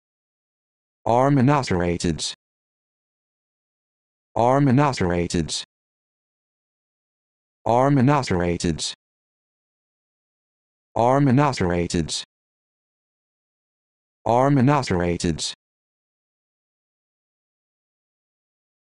How to Pronounce Armenoceratids
This video shows you how to pronounce Armenoceratids